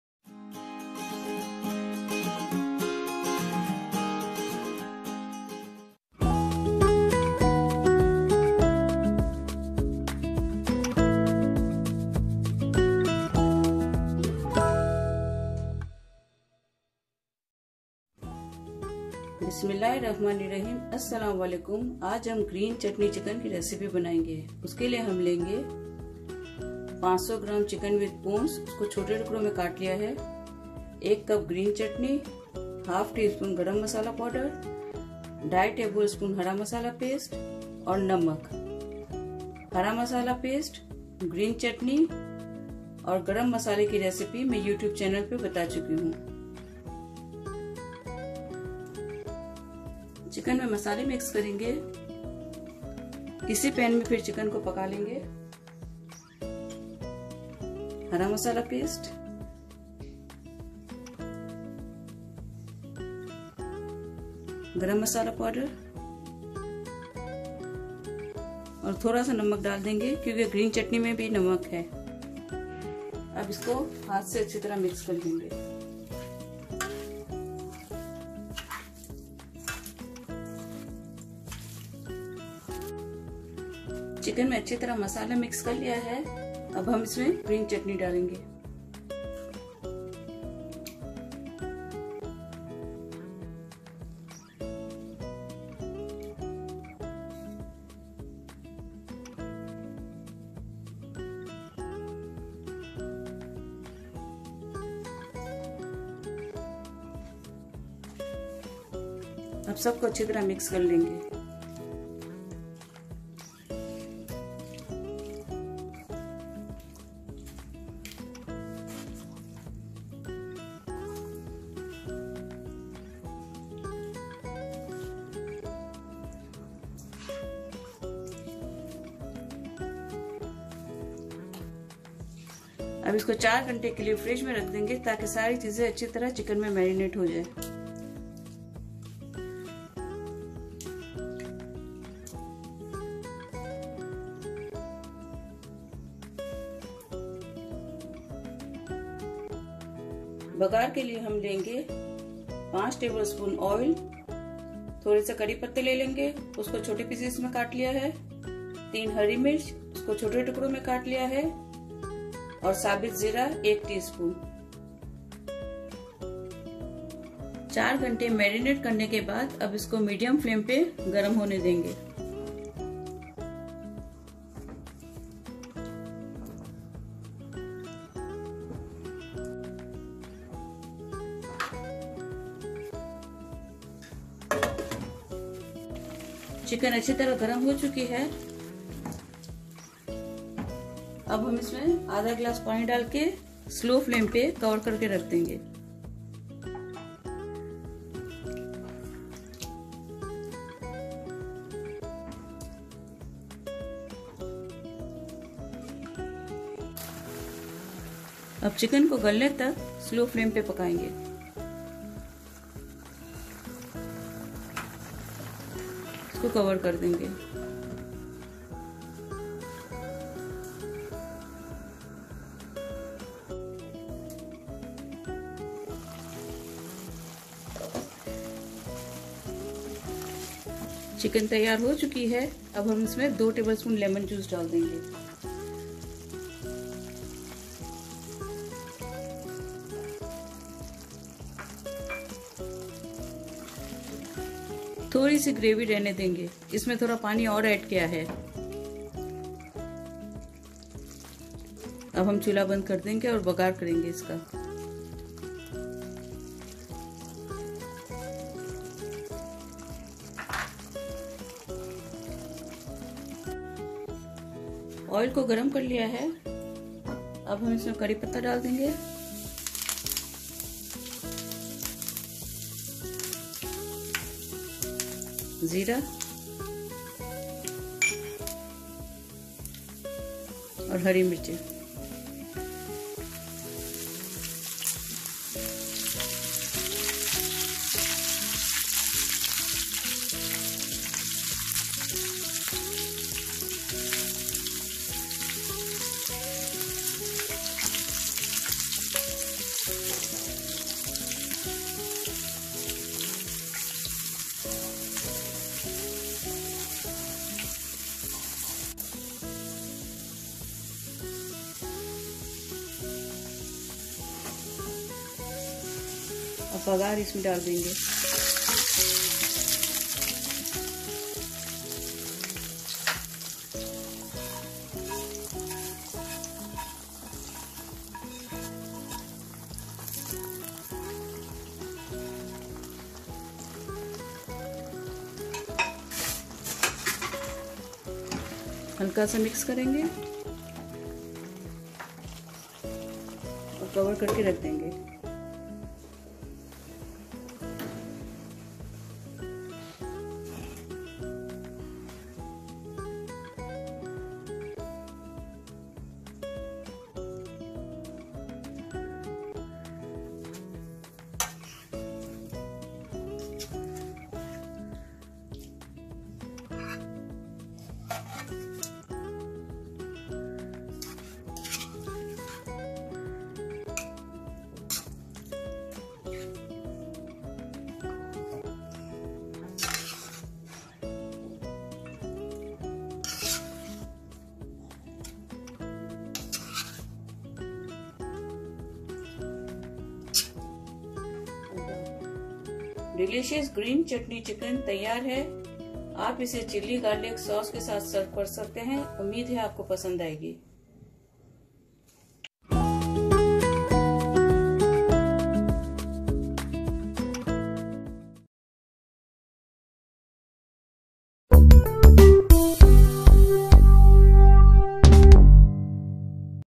Discover the Mouthwatering Flavors of Green Chutney Chicken
Movie 8.mp4. Discover the Mouthwatering Flavors of Green Chutney Chicken

TIMESTAMP

00:00 – Intro
00:17 – Ingredients
00:48 – Method
05:58 – Presentation

INGREDIENT
(MARINATION)
500 gr. chicken with bones
1 cup. green chutney
½ tsp. garam masala powder
Salt as required
2½ tbsp. Green Masala

(BAGAAR)
5 tbsp. oil
Few curry leaves
3 green chillies
1 tsp. cumin seeds

Do try this and give your feedback in the comment section below.

where we post our latest and most interesting recipes.

Want more information related to the video? You can leave your queries in the comment section below.